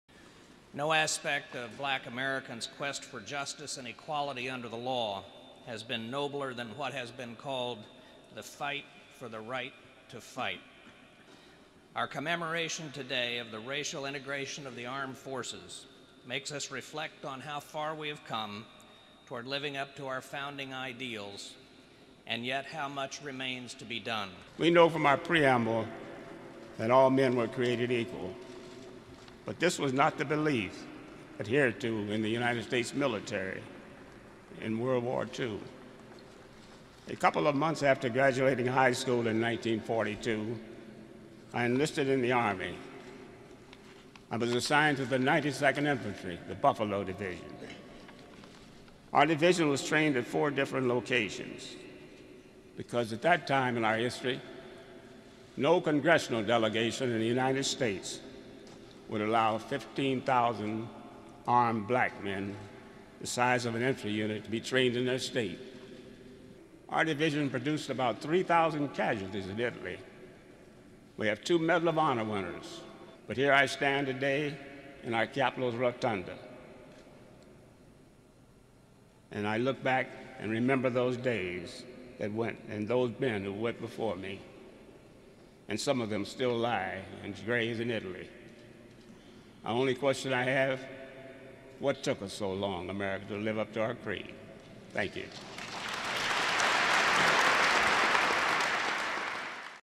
Congress Marks 60 Years of Military Integration
PlusCongress Marks 60 Years of Military IntegrationCongress Marks 60 Years of Military IntegrationThe Associated PressMembers of Congress and veterans hold an event to commemorate President Truman's executive order that marked the beginning of racial integration in the armed services. (July 23)This video contains ONLY natural sound. No script is available.